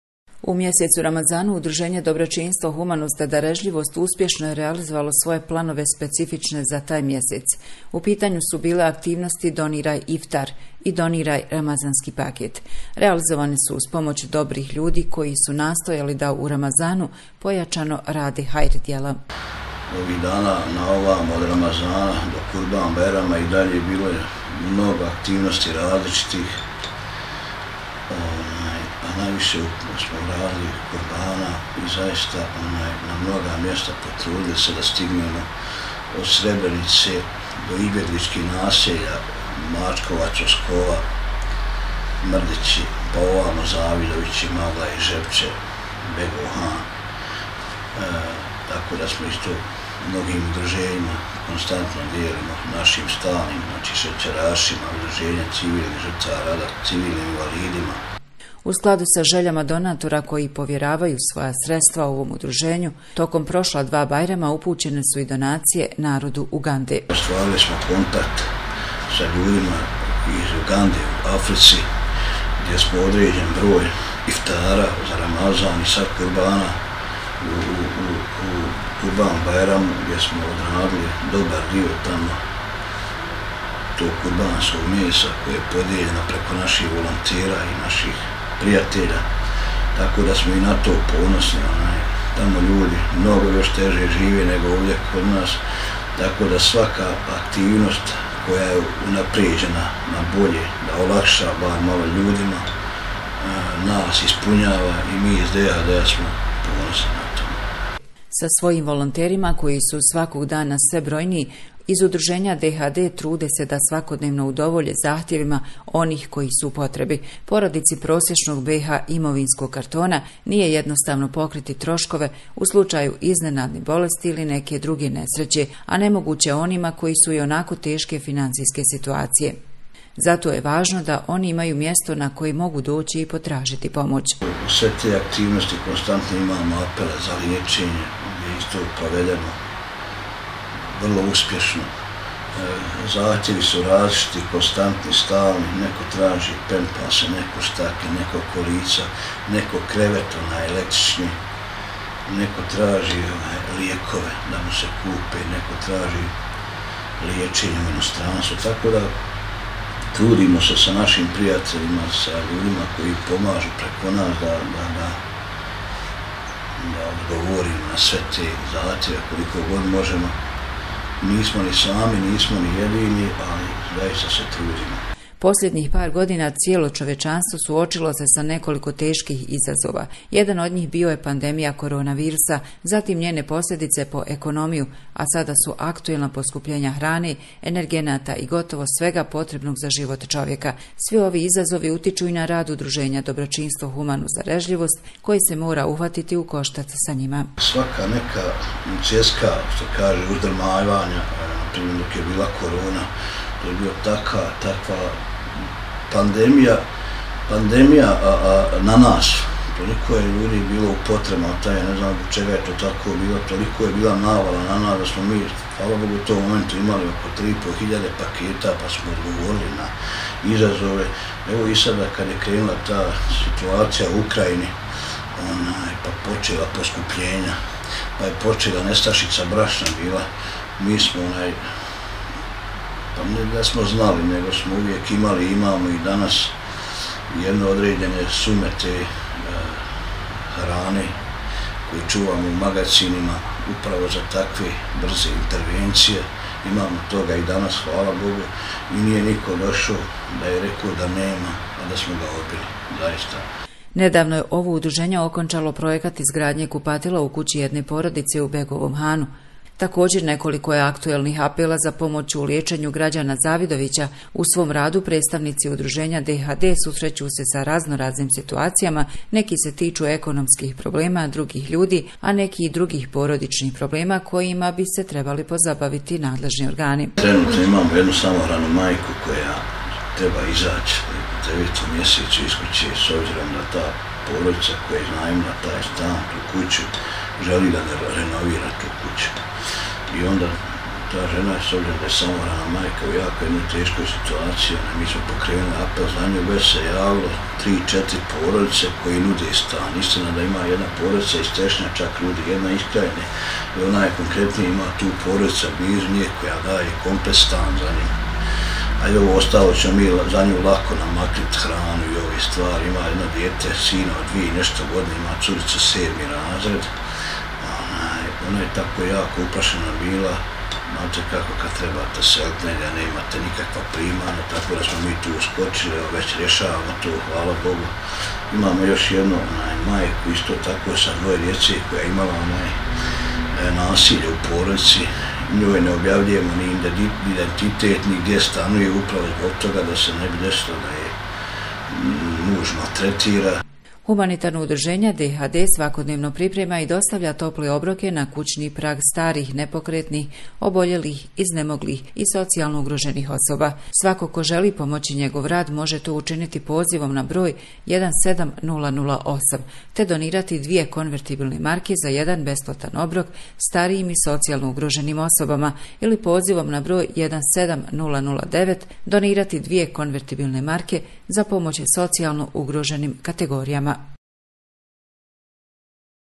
Hronika: Zavidovići 285 - Sve više zahtjeva prema Udruženju „Dobročinstvo, humanost, darežljivost“
Udruženje „Dobročinstvo, humanost, darežljivost“ djeluje na polju humanitarnog rada u Zavidovićima i šire. Trenutna ekonomska situacija uticala je na brojne porodice, pa je u posljednje vrijeme primijetan povećan broj zahtjeva za pomoć upućenih prema ovom udruženju. Sa predsjednikom udruženja Nuragom Fojnicom, razgovarali smo o posljednjim realizovanim aktivnostima.